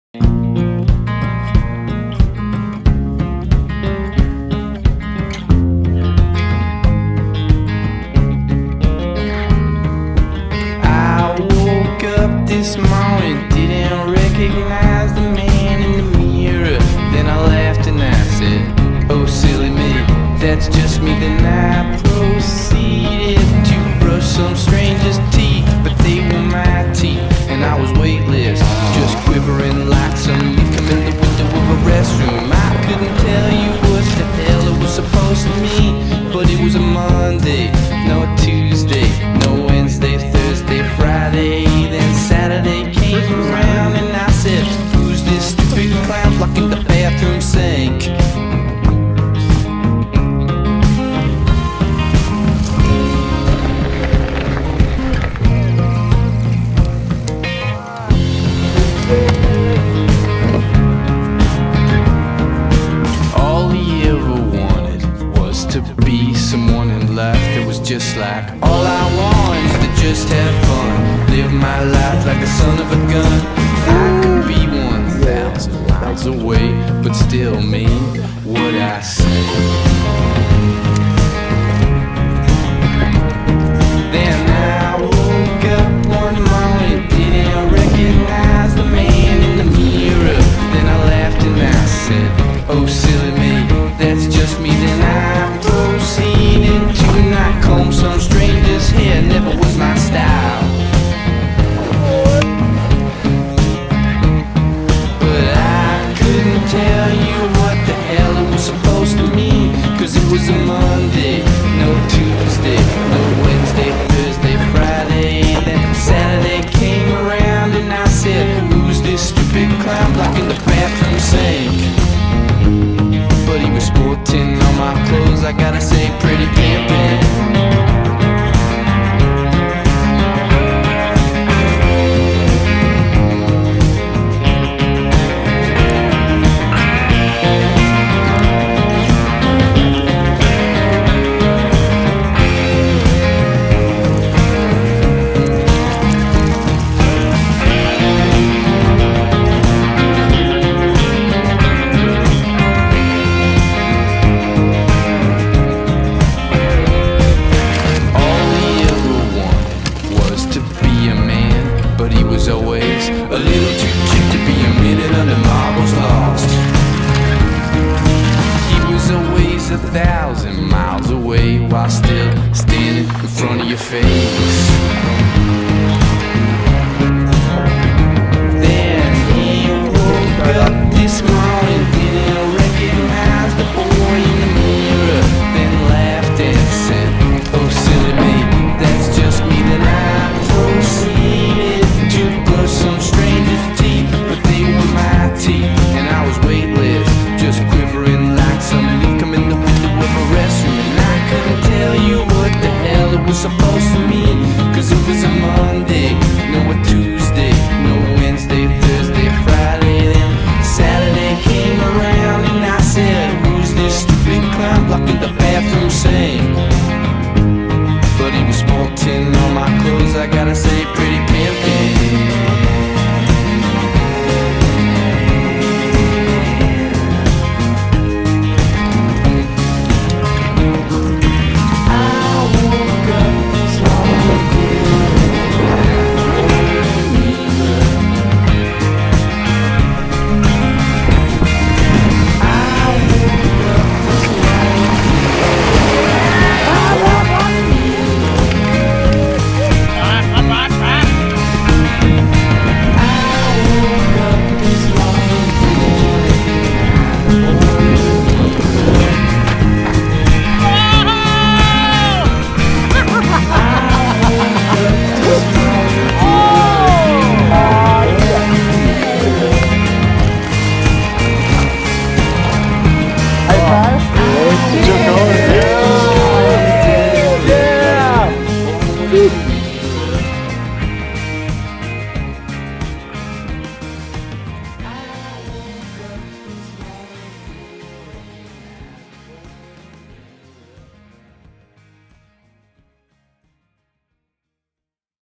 2016 Race Face Catalogue Shoot - Chilcotin's
Race Face athletes Wade Simmons, Fanny Paquette, and Audric Lacour pack their bags and head to Chilcotin Country in BC for an epic adventure and 2017 Race Face catalogue shoot